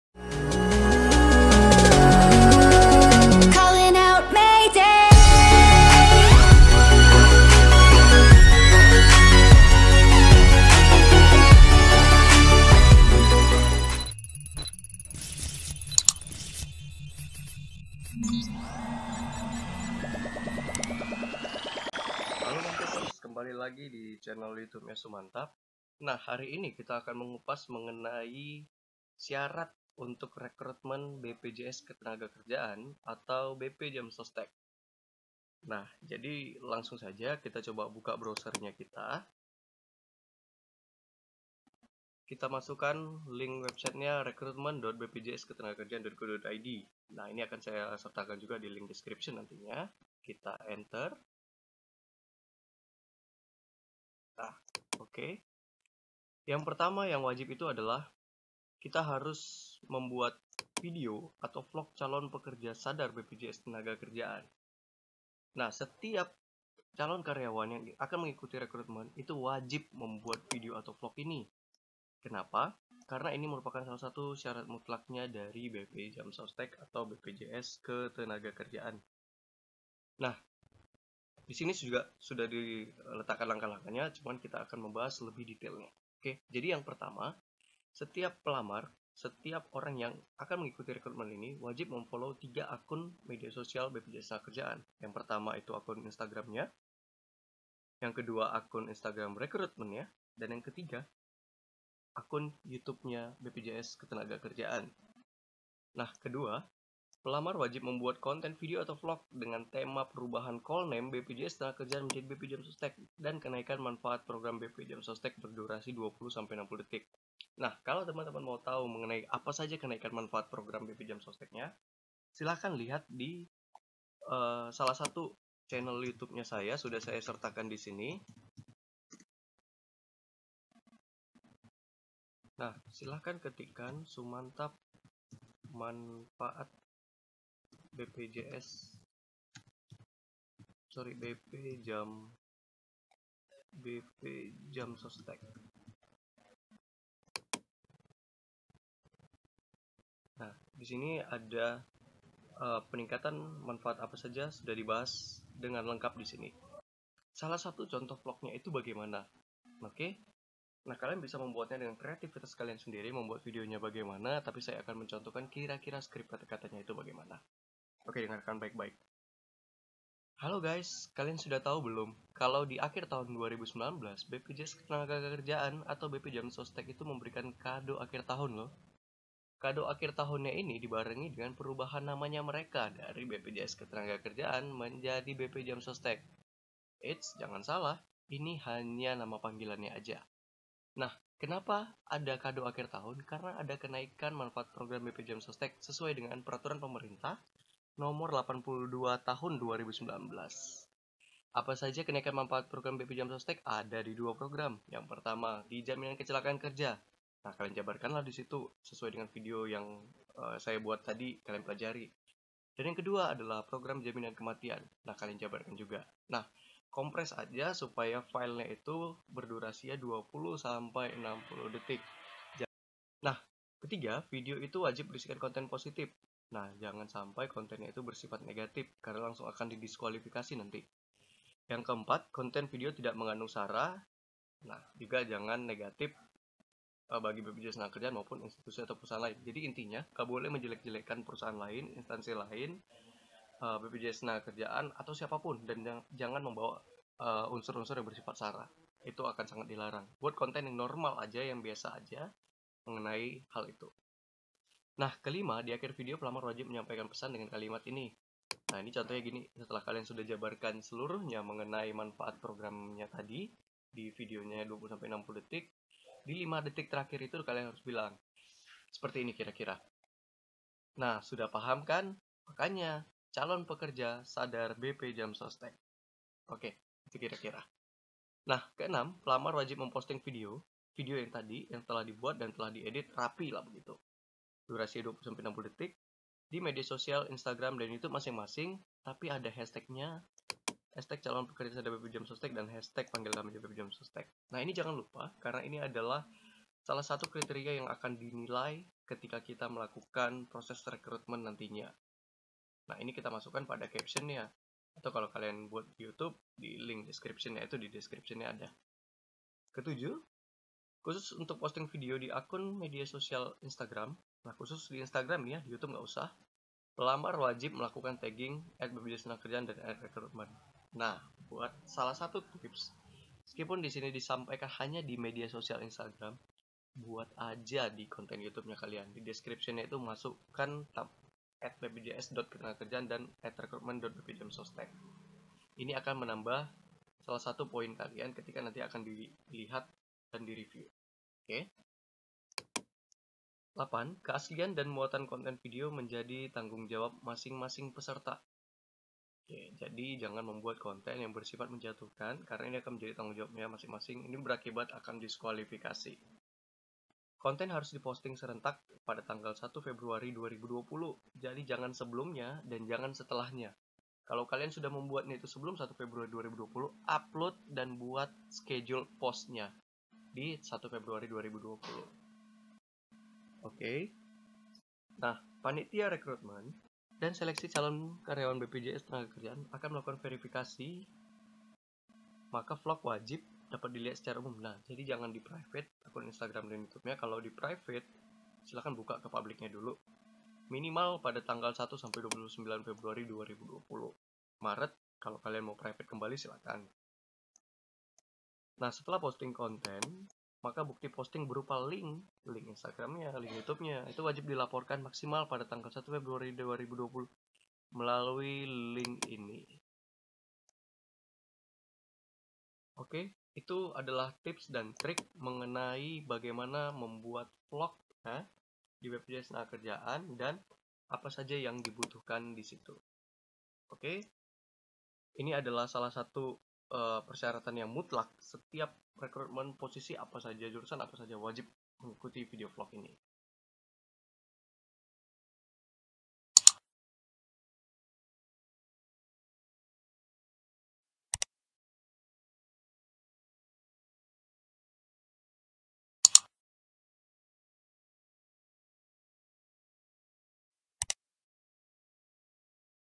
Syarat Mutlak & Wajib Tau! Tips n Trick Membuat Vlog Rekrutmen BPJAMSOSTEK (BPJS Ketenagakerjaan)
Halo mantaperz, mau ikut seleksi / rekrutmen BPJAMSOSTEK (BPJS Ketenagakerjaan)?